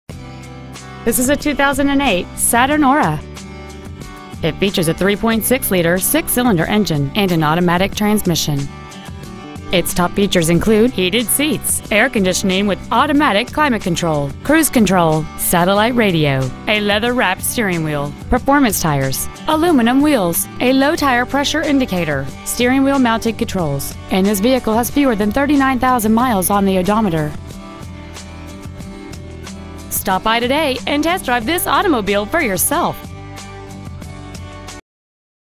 2008 Saturn Aura Gurnee IL
We are proud to present this 2008 Saturn Aura.

 Year: 2008
 Make: Saturn
 Model: Aura
 Engine: 3.6L V6
 Trans.: Automatic
 Exterior: Blue
 Miles: 38,195
 Interior: Brown